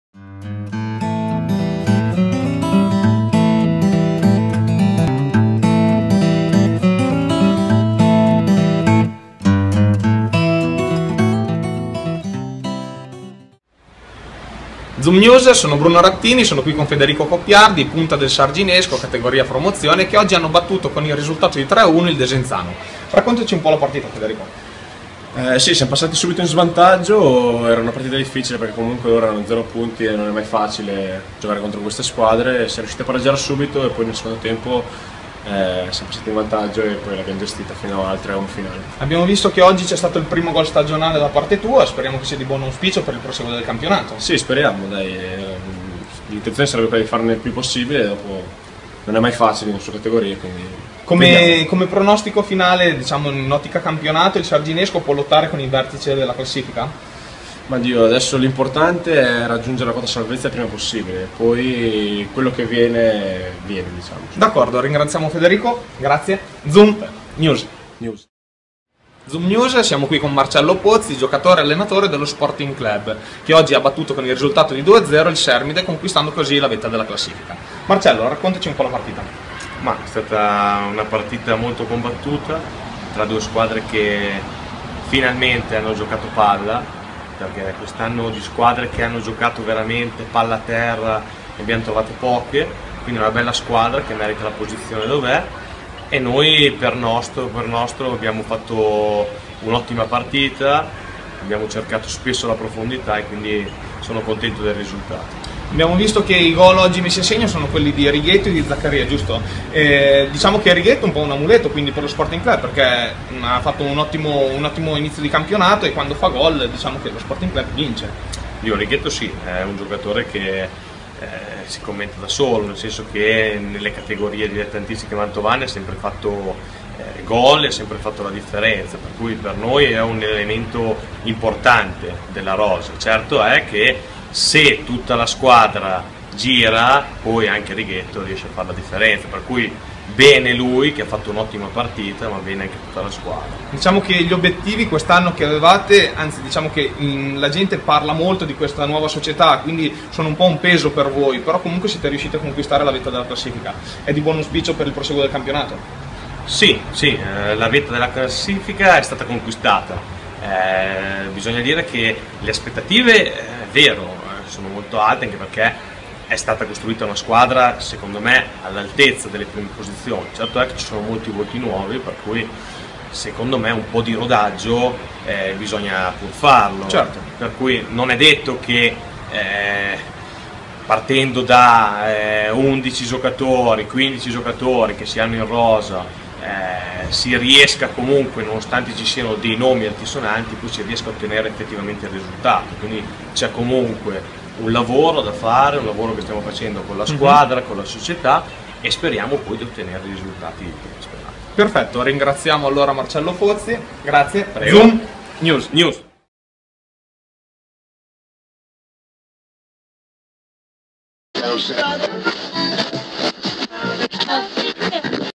Intervista a Coppiardi e Pozzi
Il nostro inviato Bruno Rattini ha intervistato Federico Coppiardi del Sarginesco e Marcello Pozzi dello Sporting Club.